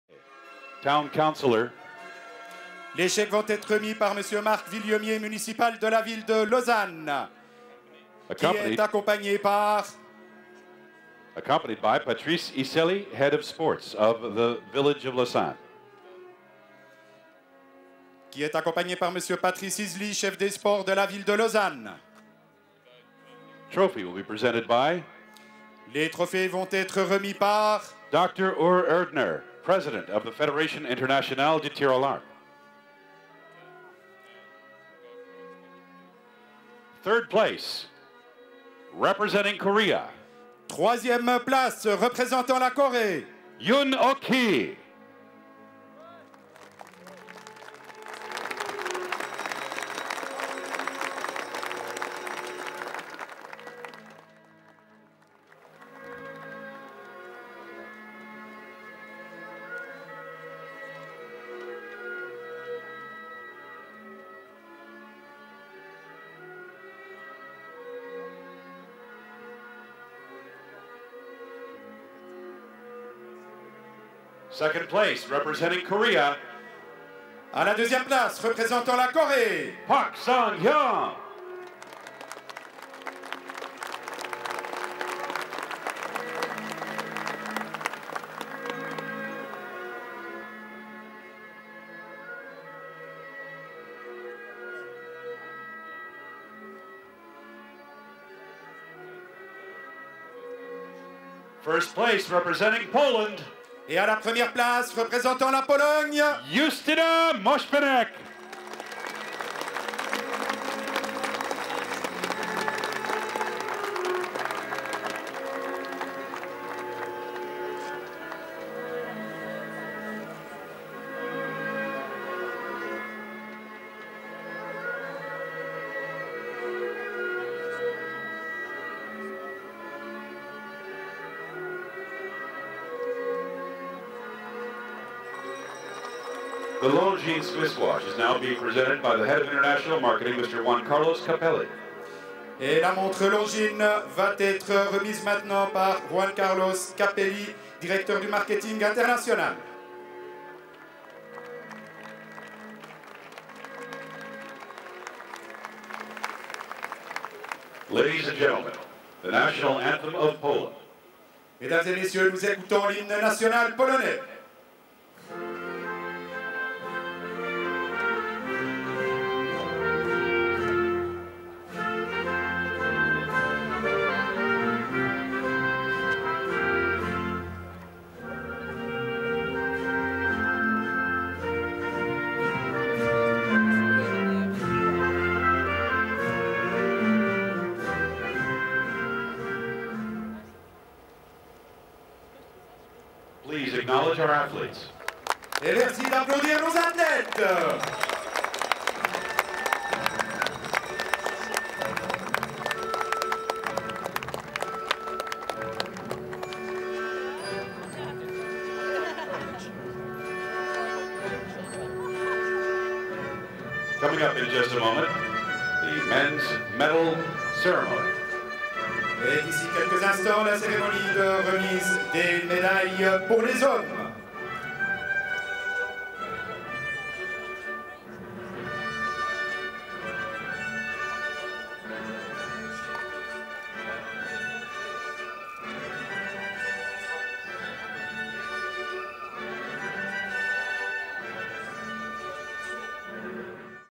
Archery World Cup 2008 - FINAL STAGE- Ind. Podium
Uncut award Ceremony of the final Stage of the 2008 Meteksan Archery World Cup in Lausanne(SWITZERLAND).
Women Individual Recurve Podium